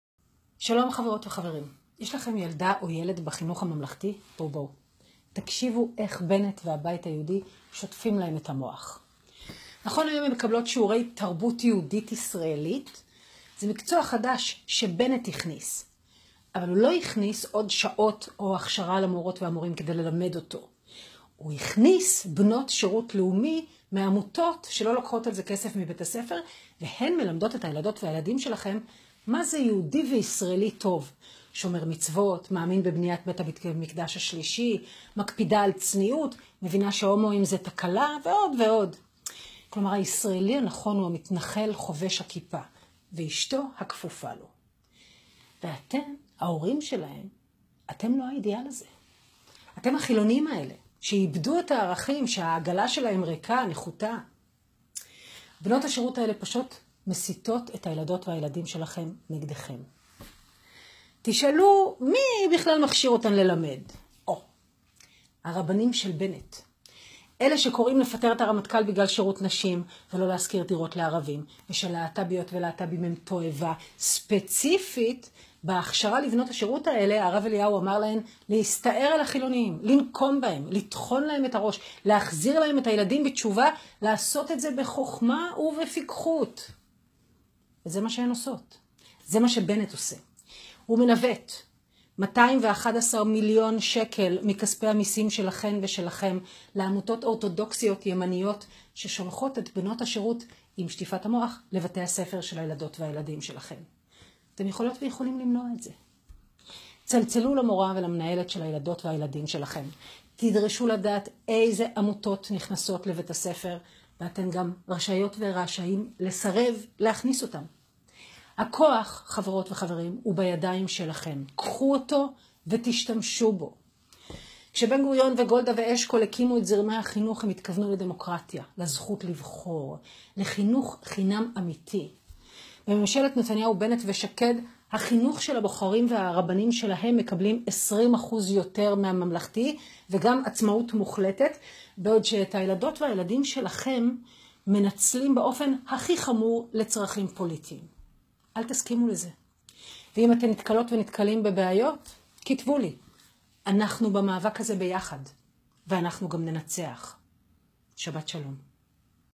ח״כ מרב מיכאלי להורי ישראל: ״בנות השירות מסיתות את הילדים שלכם נגדכם״
השר נפתלי בנט

הצטרפו אלינו Join Us